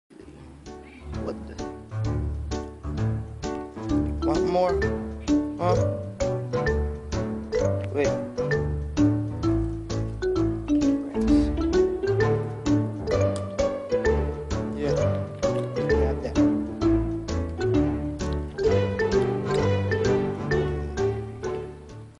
Cat begging for crackers
This is my cat who is being fed by her hooman with crackers 😂